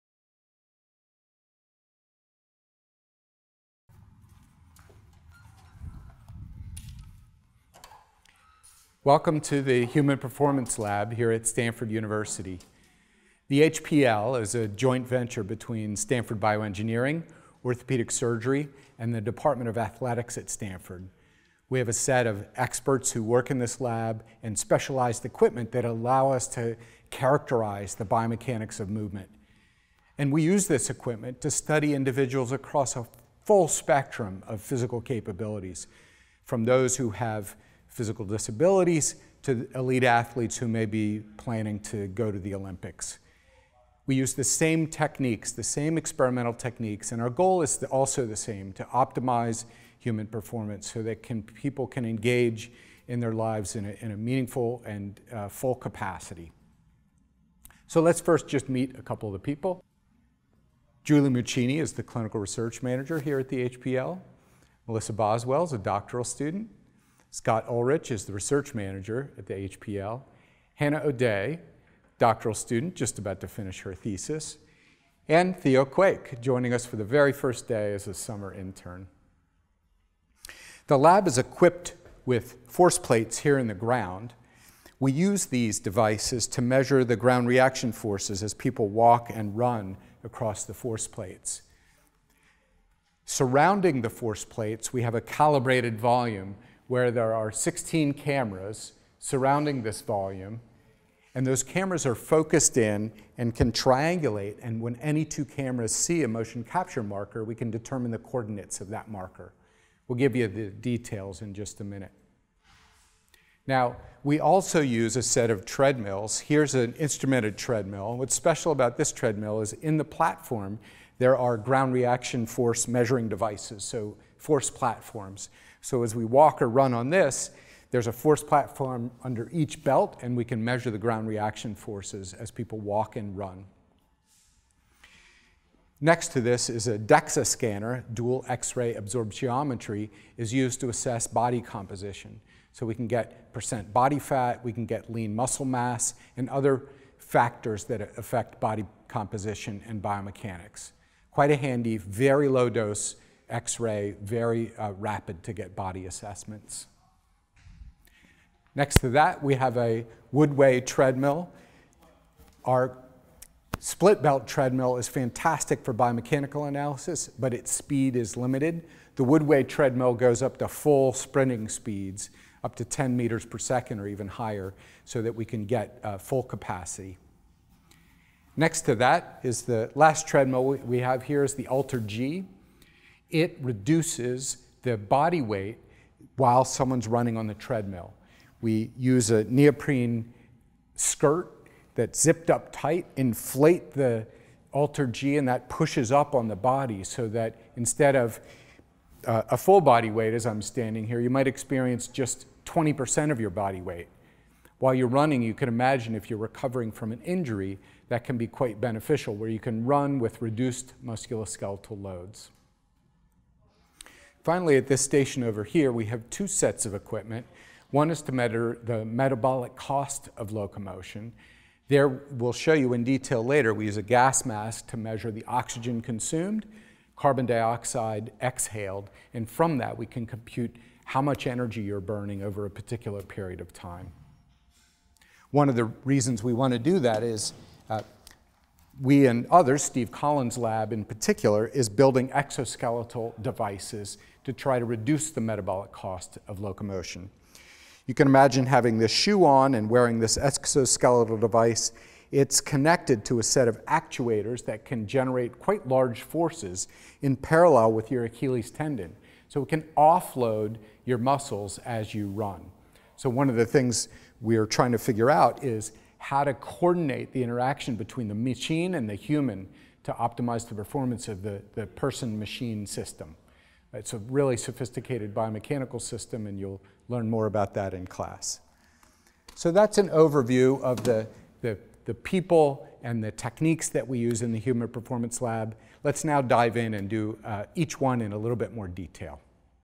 Biomechanics of Movement | Demo: Welcome to Stanford's Human Performance Lab
Professor Scott Delp welcomes you to the Stanford Human Performance Lab (HPL), a joint venture between Stanford Bioengineering, Orthopaedic Surgery, and Athletics. Scott gives a tour of the HPL and the equipment used to run the many research studies supported by the HPL whose goals are to optimize human performance. Key equipment includes force plates, various treadmills, a DEXA scanner, and a system to measure the metabolic cost of locomotion.

Acknowledgments:
Clio Delp, Sebastian Kleppe, University of Ottawa (Video Production)
Marissa Lee, Melissa Boswell, Hannah O'Day (Content Review)
The Stanford Human Performance Lab especially Scott Uhlrich & Julie Muccini (Demos)
The University of Ottawa and Neuromuscular Biomechanics Lab of Stanford University